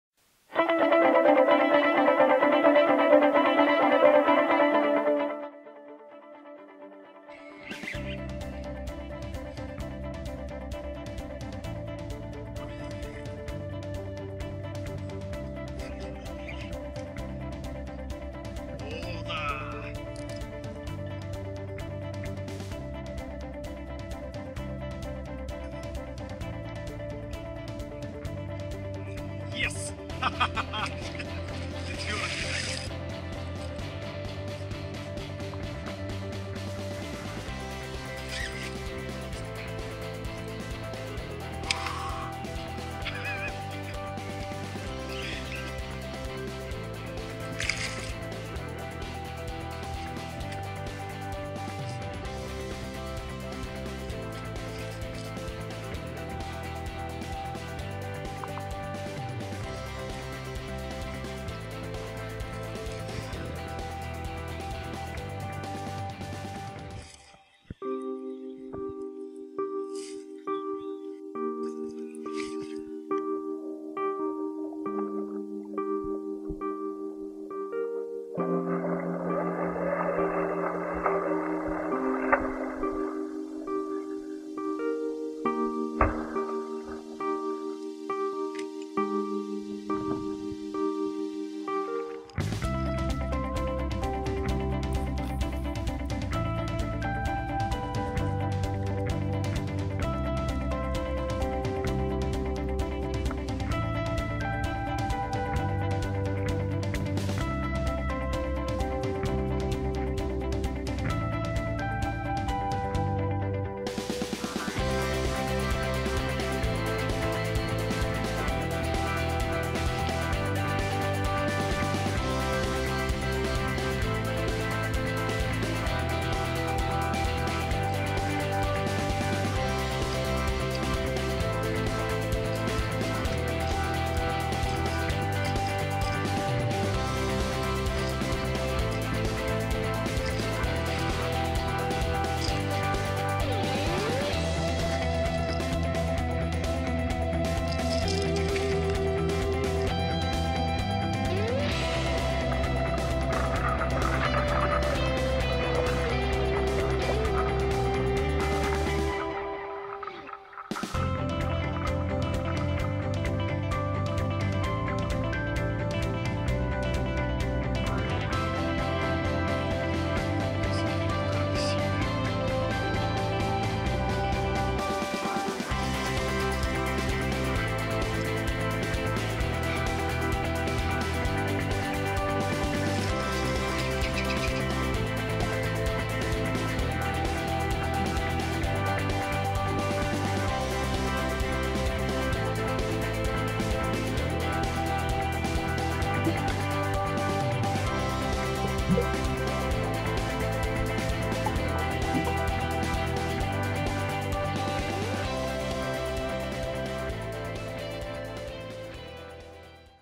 Смотри и ты не поверишь, но такое случается на рыбалке! 釣り Обидные сходы щуки
Ждёшь поклёвки крупной рыбы. И вот она, огромная рыбина на крючке, но.
Видео - красивая рыбалка, клёв просто улет, рыбы просто огромные) + смешные моменты .Супер видео Io как вам эта рыбалка ОЦЕНИТЕ, но а щук мне тоже жалко)))